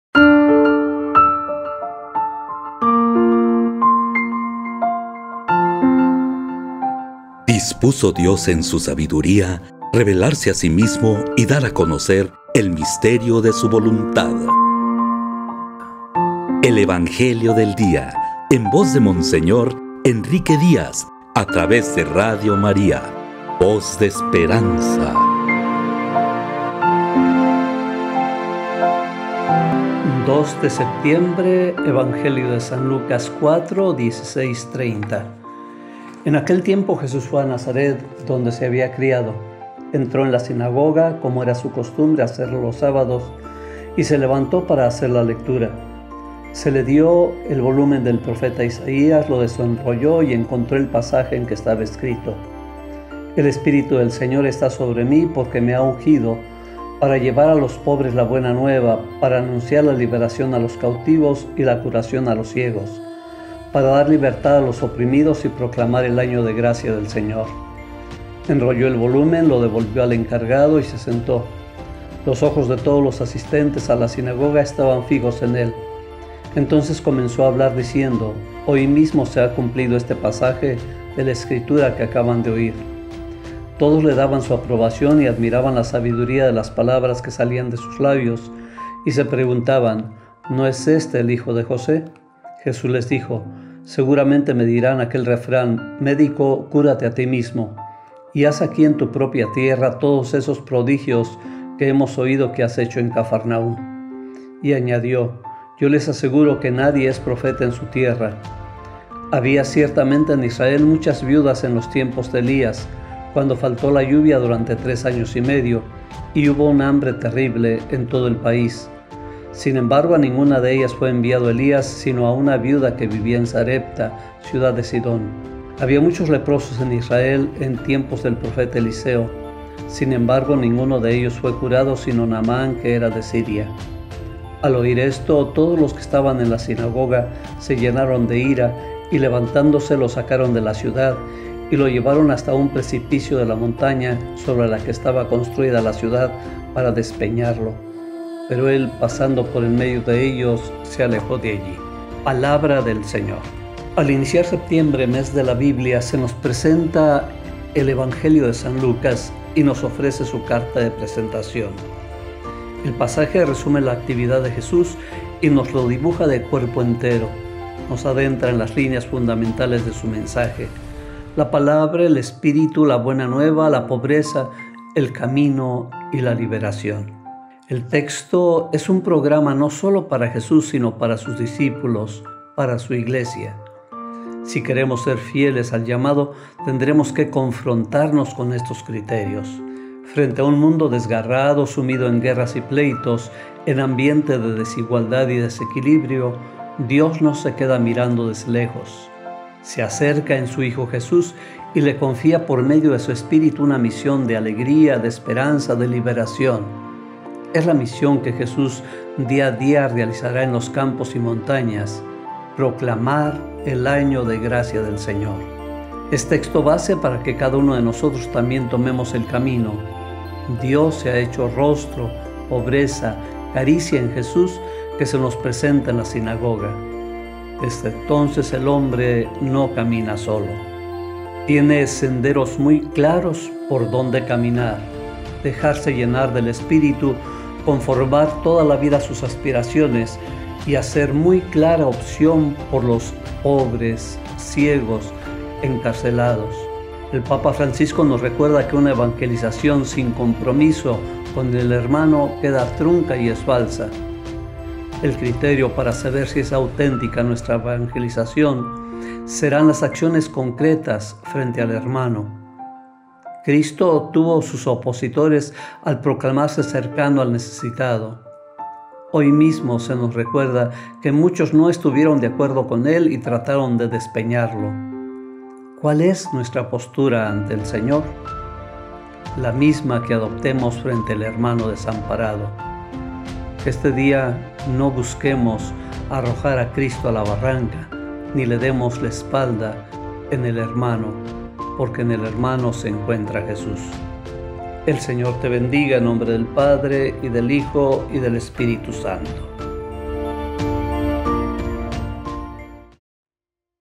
El Evangelio de Día | Monseñor Enrique Díaz | 2 Septiembre 2024
San Lucas 4, 16-30: “Me ha enviado para llevar a los pobres la buena nueva – Nadie es profeta en su tierra”